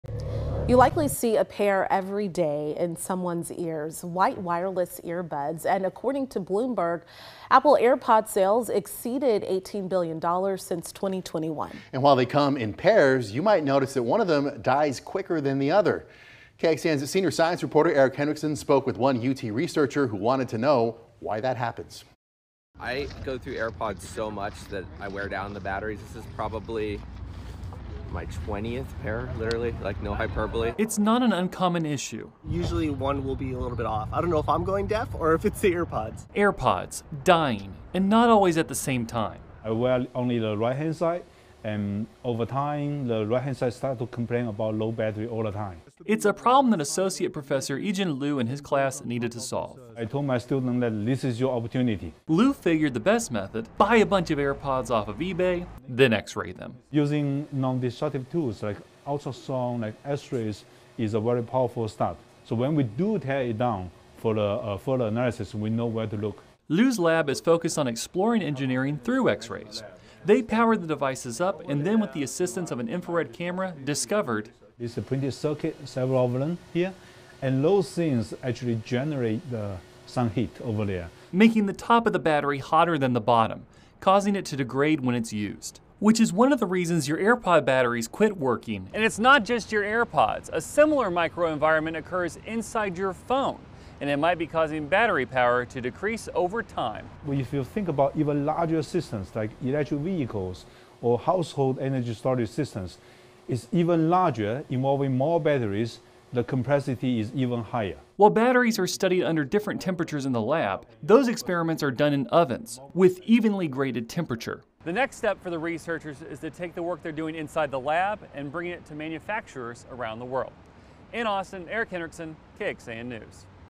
AirPods dying? UT professor discovers microenvironment that may be responsible
A tiny microenvironment in your wireless earbuds may be the reason they're dying so quickly, according to new research from the University of Texas.